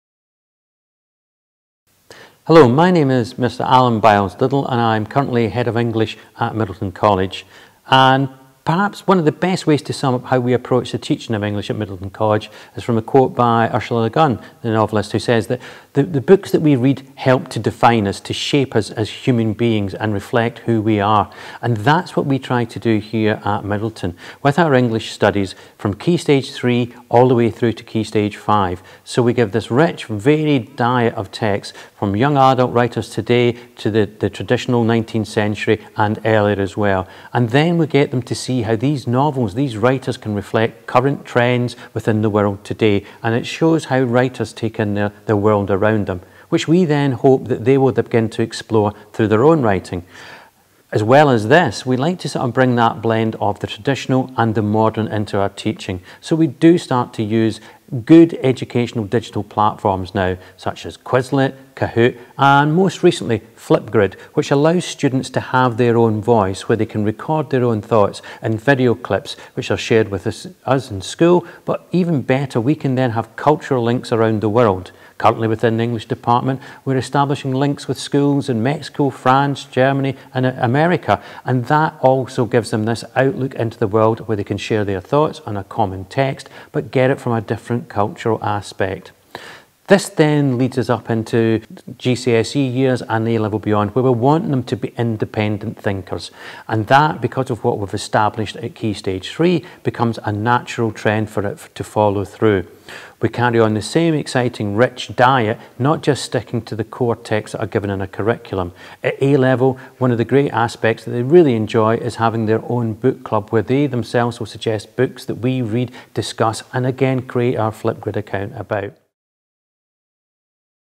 English at Myddelton College
Meet Mr Alan Biles-Liddell, our Head of English